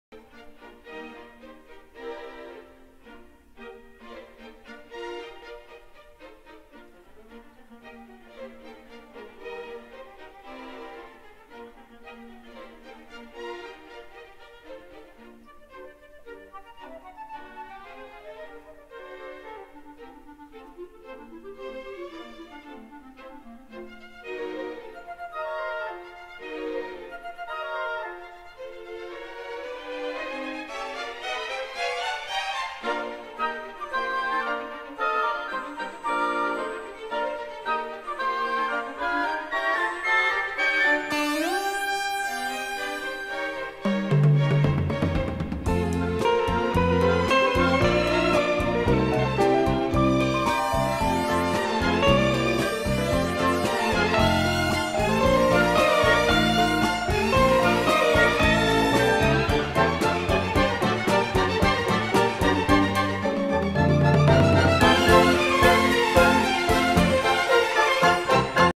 Nutcracker Fantasy 1979 rare Photos!
Most photos are of the display dolls them selves and then one that features all of the dolls but the ragman